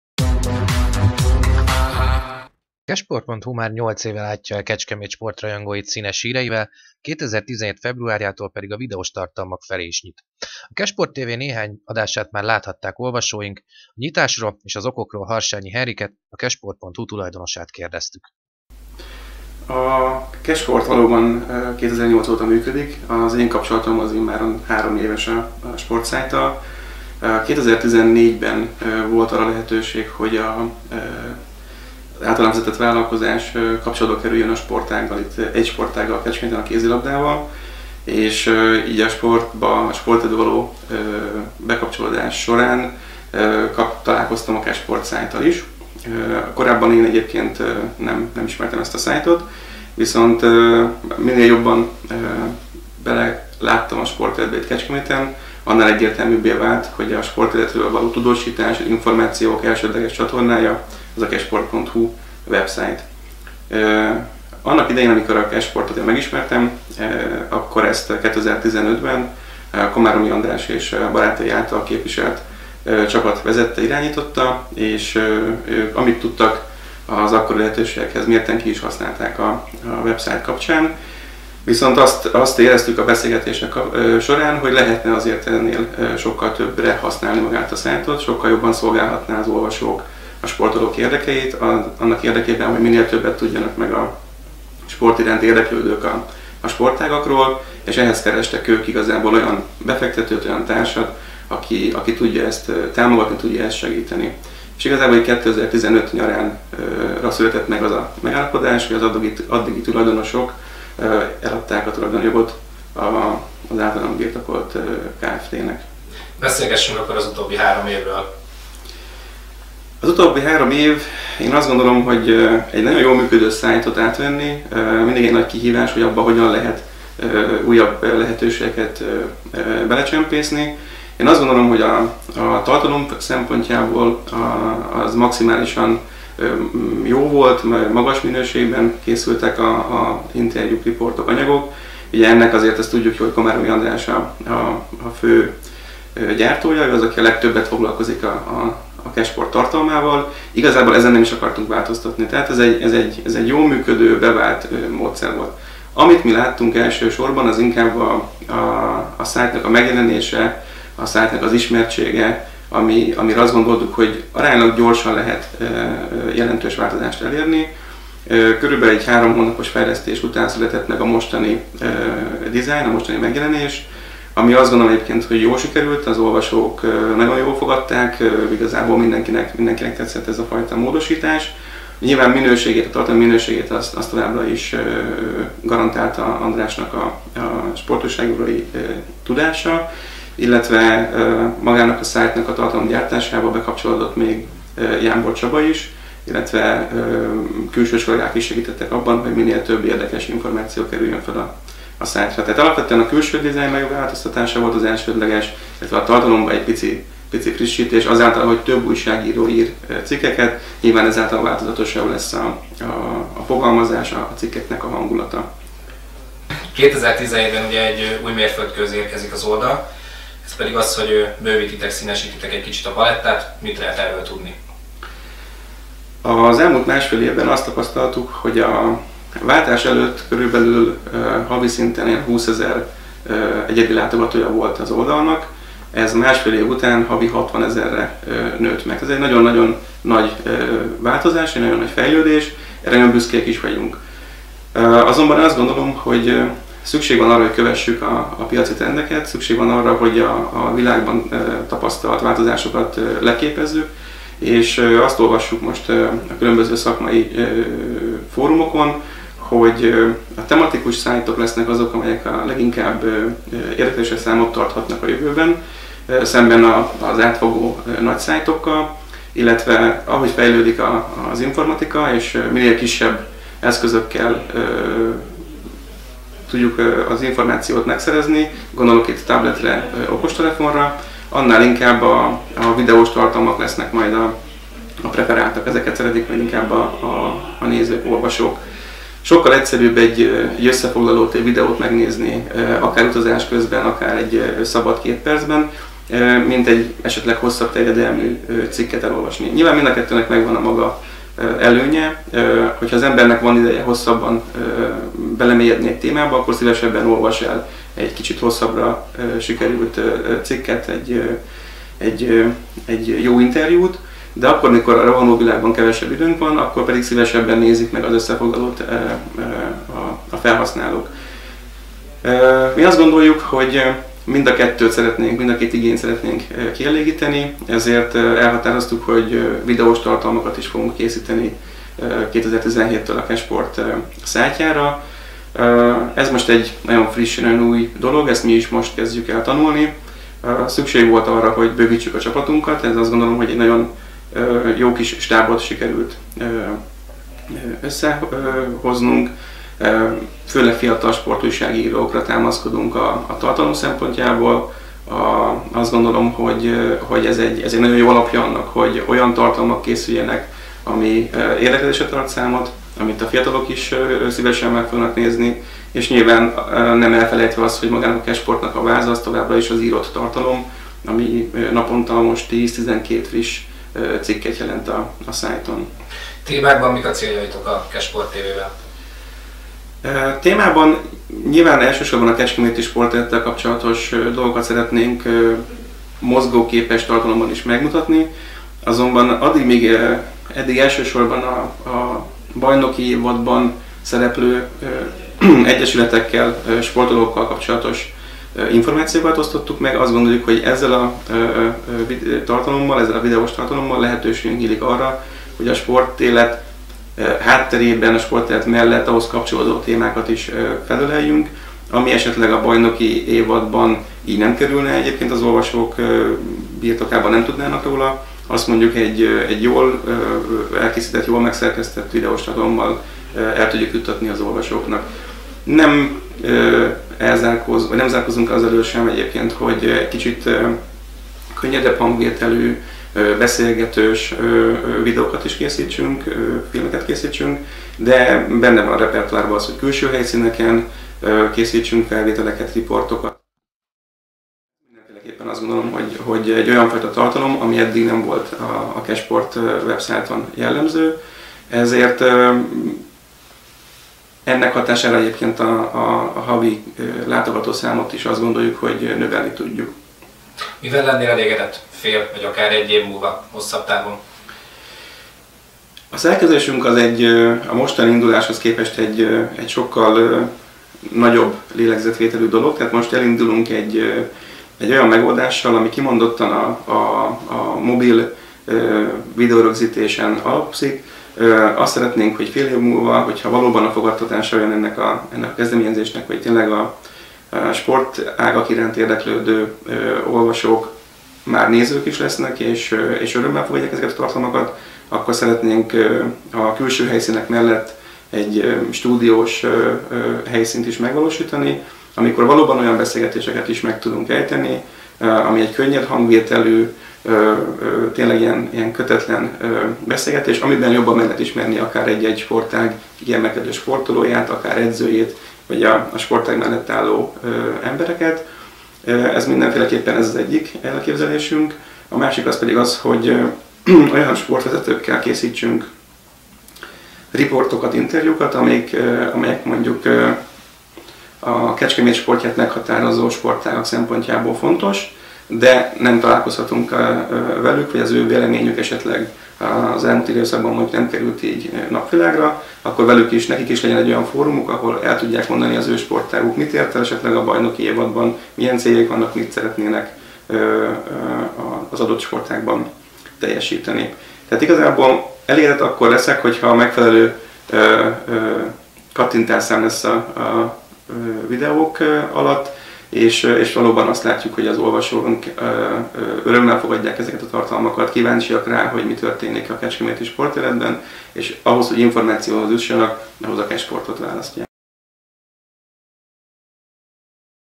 A videós tartalom felé nyit a Kesport, elindult a Kesport TV!
2017 februárjától új területek felé is nyit a kesport.hu, ugyanis immáron saját videós tartalmakkal is jelentkezik oldalunk. A bővülő stábról, a célokról és a okokról a kesport.hu tulajdonosa, Harsányi Henrik beszélt.